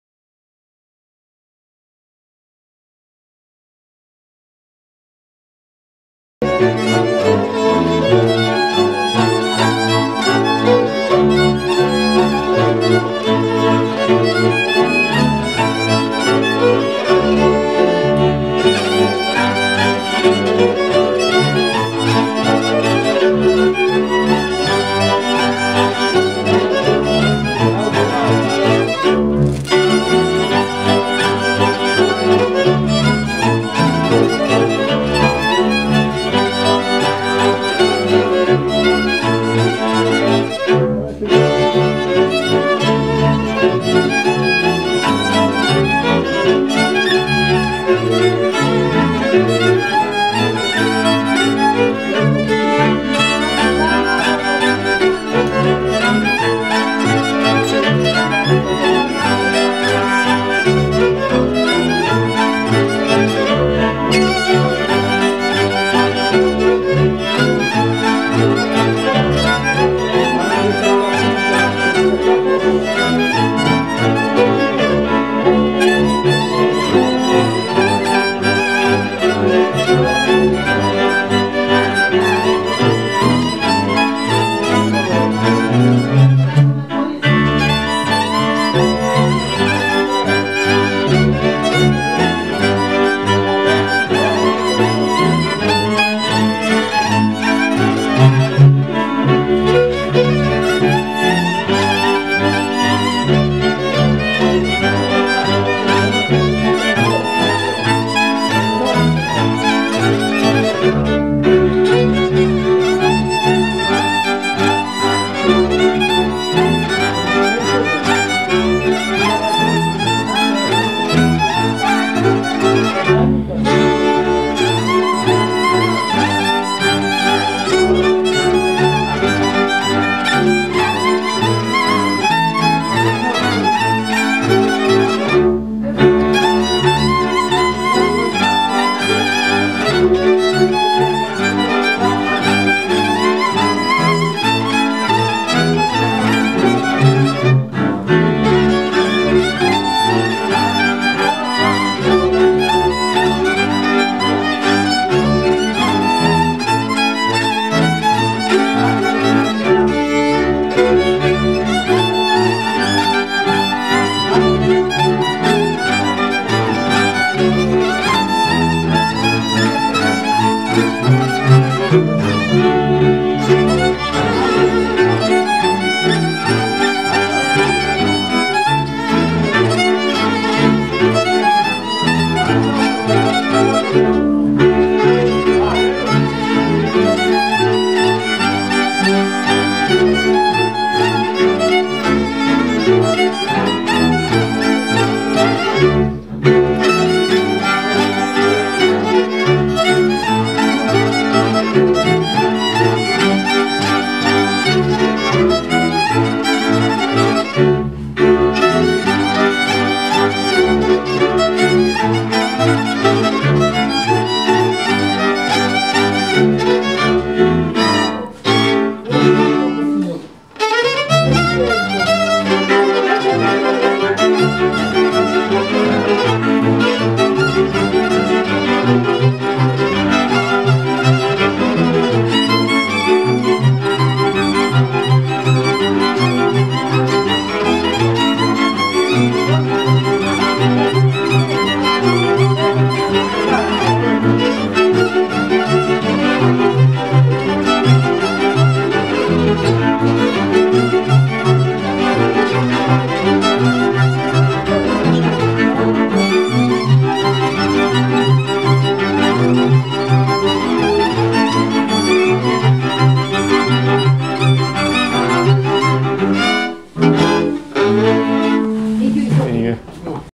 Szászcsávás: Mezei Levente "Leves" (1969-2017) plays Invirtita and Hartag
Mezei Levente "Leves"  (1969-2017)
with Mezei Alin - contra
Mezei Levente - double bass

Invirtita and Hartag

Recorded at a pub in the village of Csávás
Romania

summer 2007